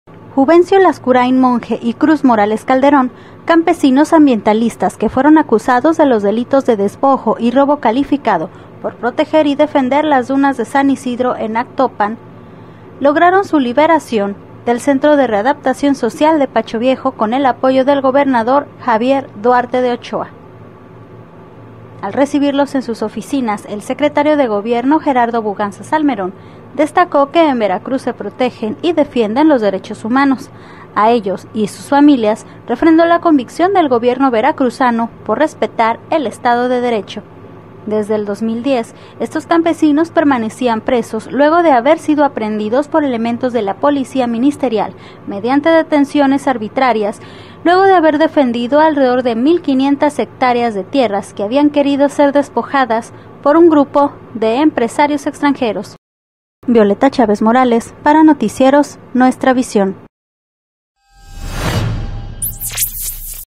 Libres ecologistas. Por intervención del gobierno
Campesinos ambientalistas que habían sido detenidos el 2010, fueron liberados luego de casi 3 años de permanecer presos, tras intervención del gobierno estatal